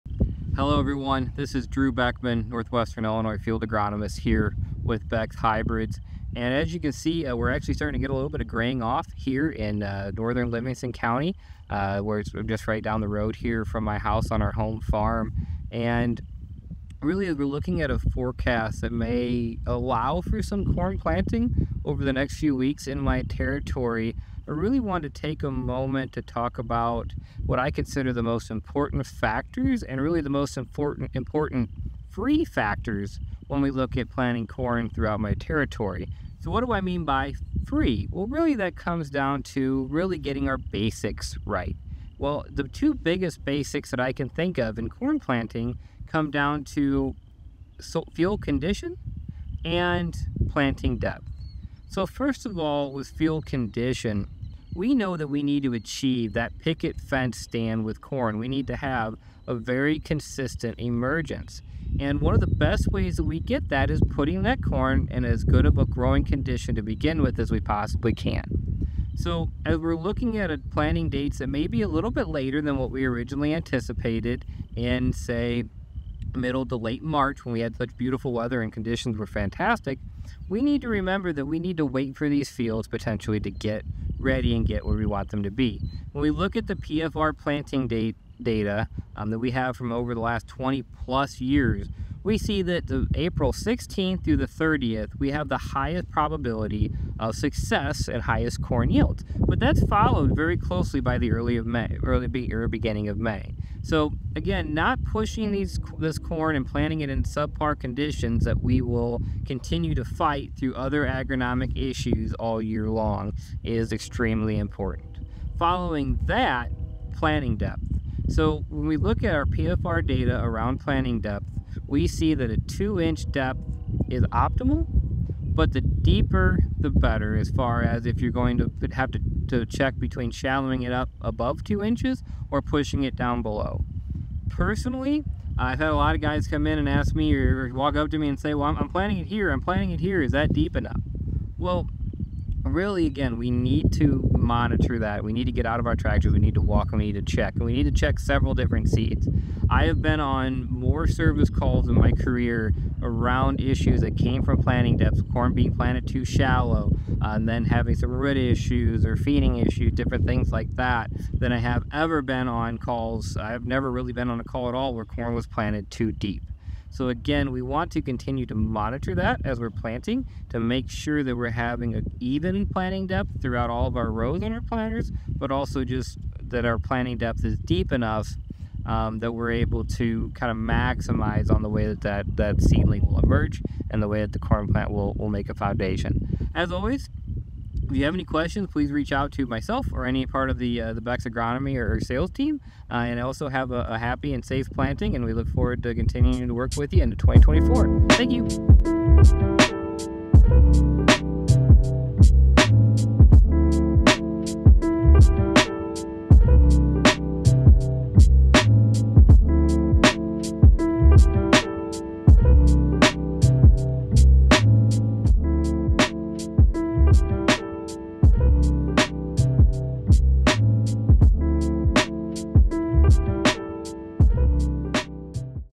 Free Factors of Corn Planting | Beck's Agronomy Update
Maximize your corn yield potential this season by focusing on the "free" factors of corn planting! In this agronomy update, Beck's Hybrids Field Agronomist, Drew Beckman, highlights the two most critical aspects of successful corn establishment: soil condition and planting depth.